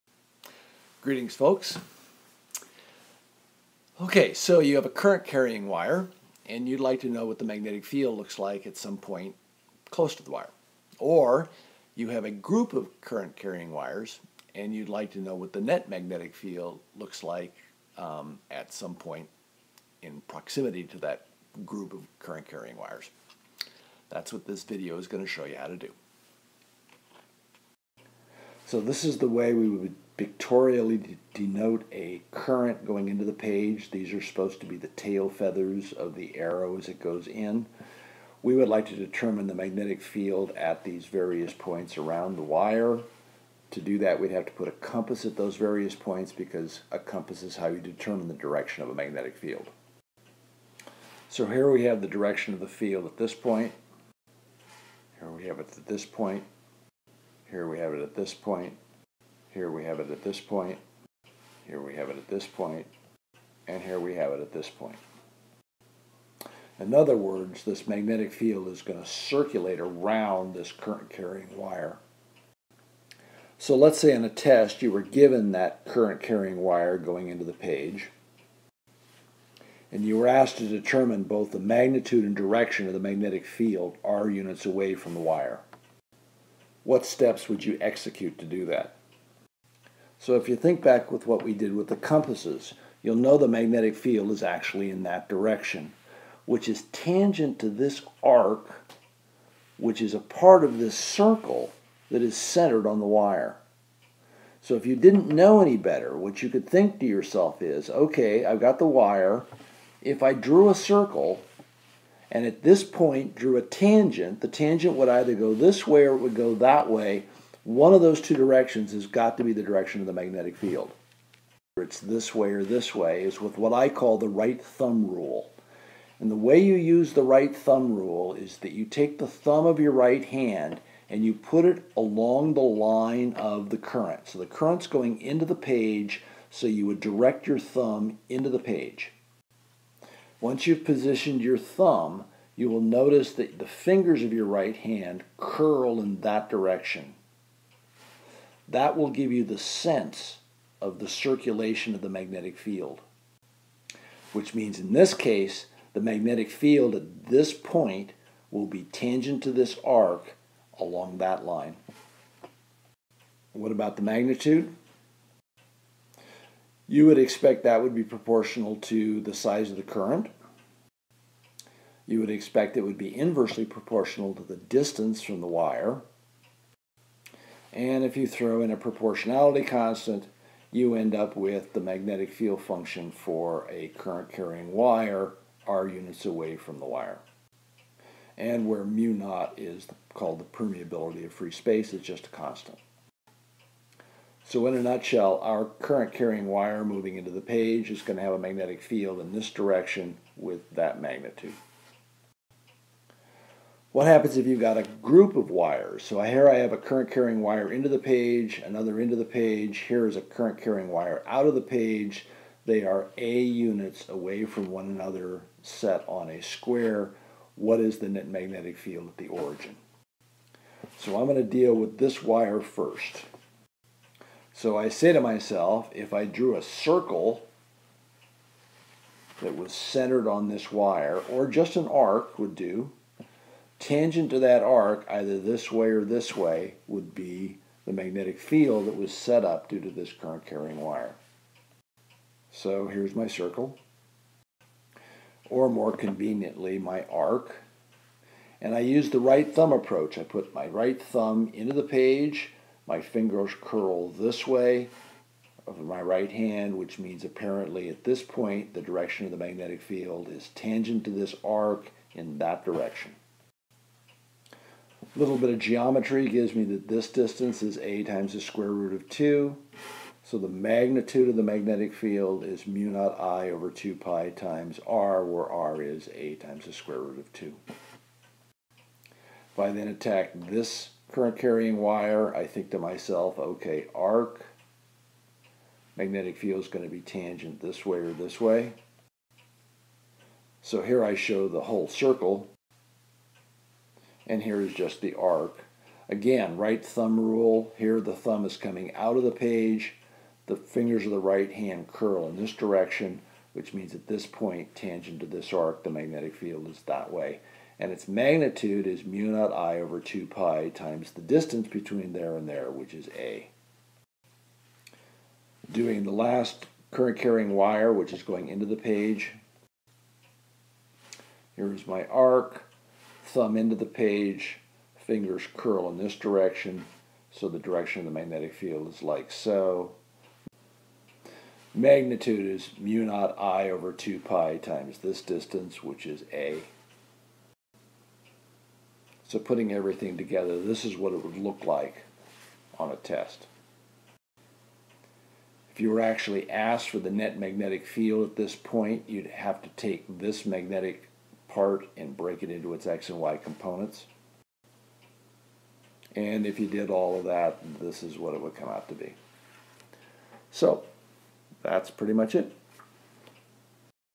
zPoly: 43 (B-fields and current carrying wires)
This is a video that identifies how to determine the direction and magnitude of a magnetic field at some point in the vicinity of a current-carrying wire.  It also looks as a situation in which there are multiple wires.